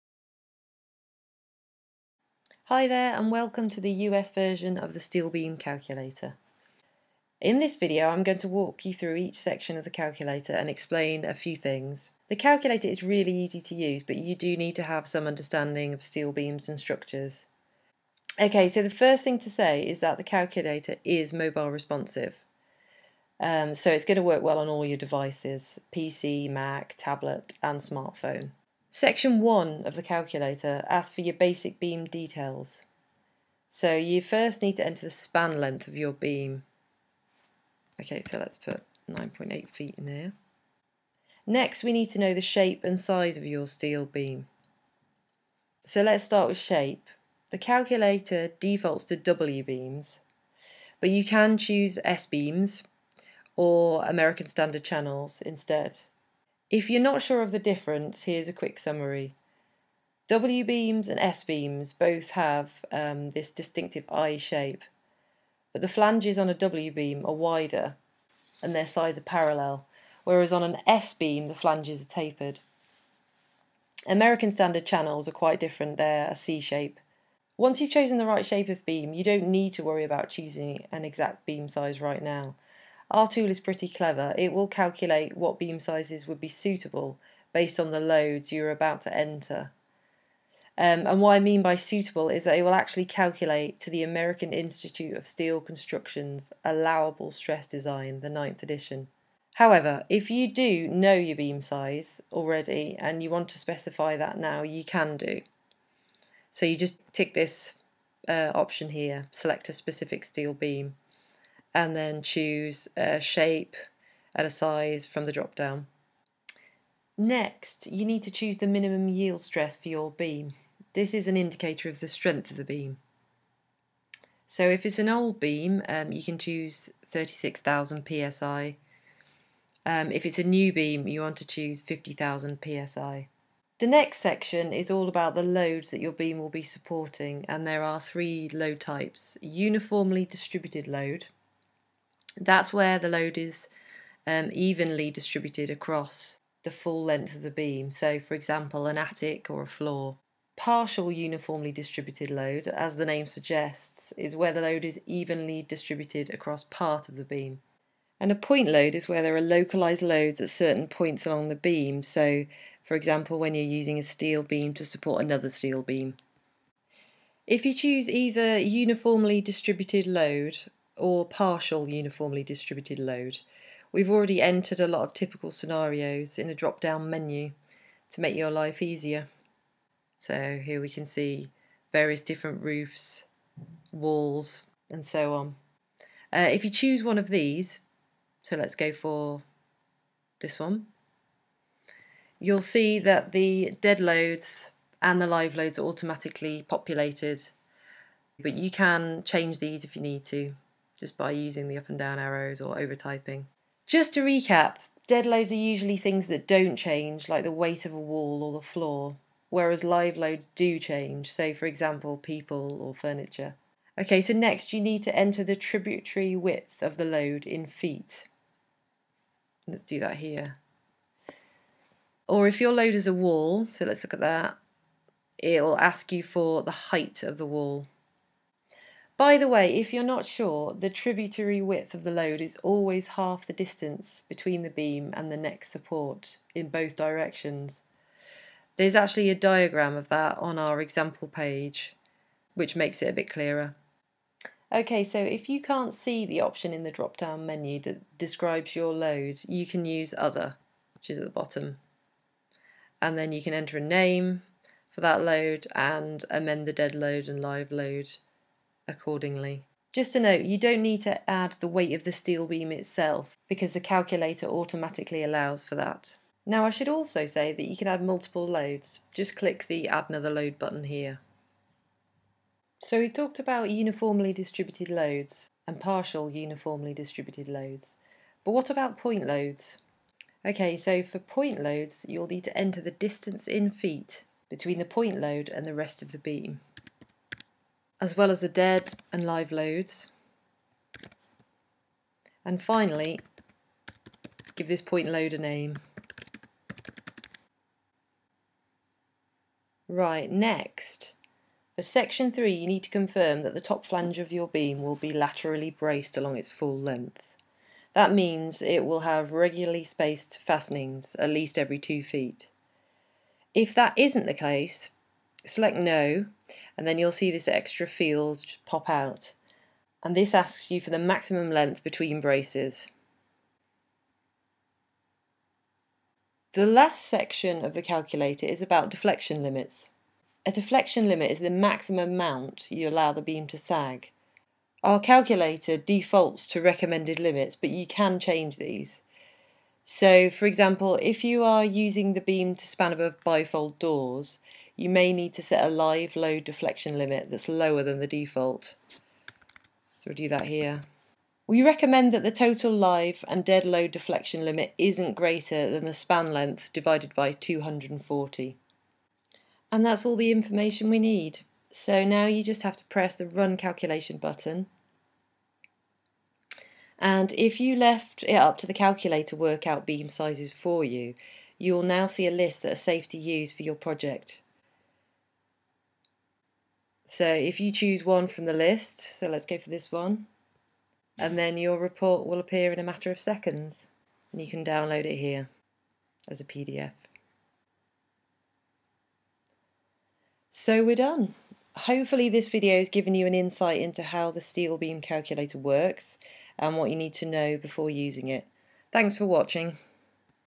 How to use steel beam calculator
A guide on how to use an online US steel beam calculator